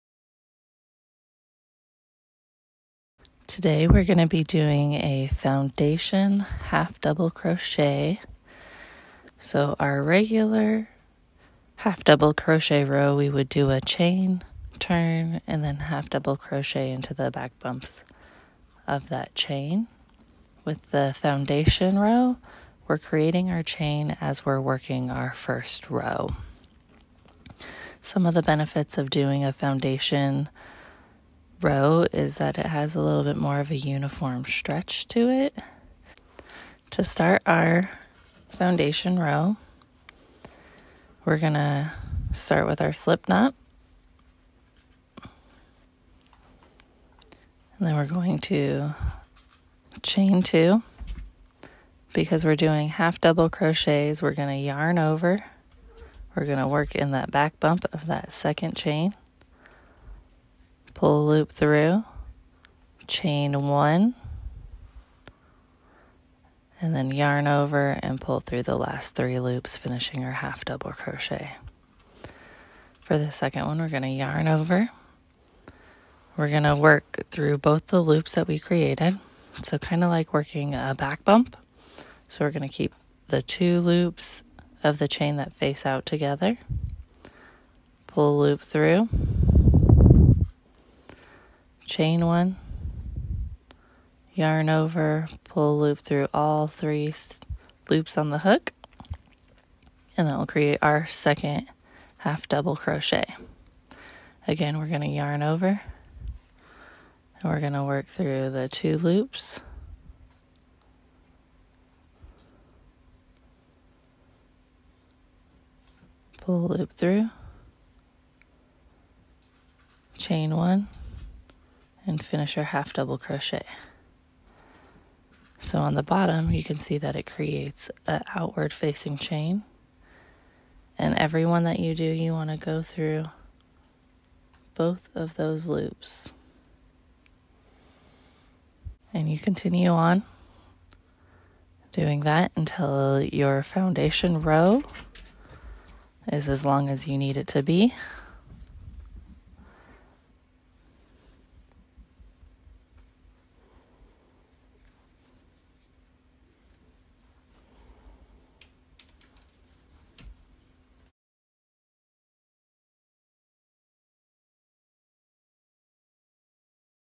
Foundation half double crochet (Fhdc) Crochet Tutorial
This is a quick crochet tutorial on how to do a foundation row with half double crochet stitches.

Here I used an Acrylic worsted weight yarn and a 4.00mm hook. As this video was made to accompany my simple cowl crochet pattern.